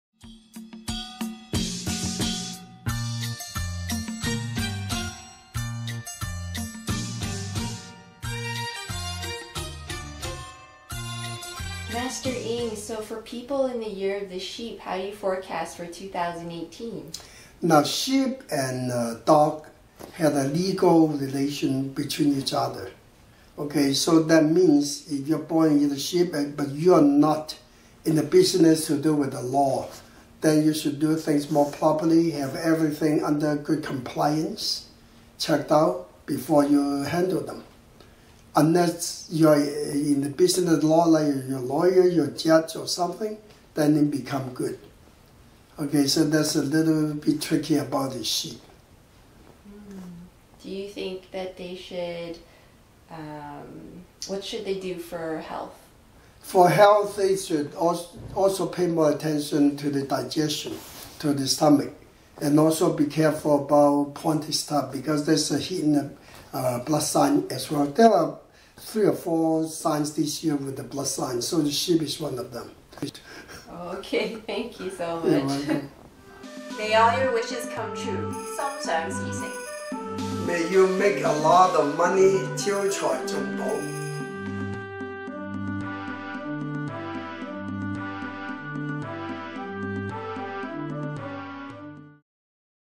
Master Paul Ng 2018 Year of Earth Dog Predictions-Sheep
Sheep-2003\1991\1979\1967\1955\1943\1931
The sheep has conflicts with the dog. There could be arguments and controversies and even chances for lawsuits. Lucky though, there is a money sign that can help out. Strong intuition touch and there is a inclination towards arts. Enjoy solitude .
Predictions by Master Paul Ng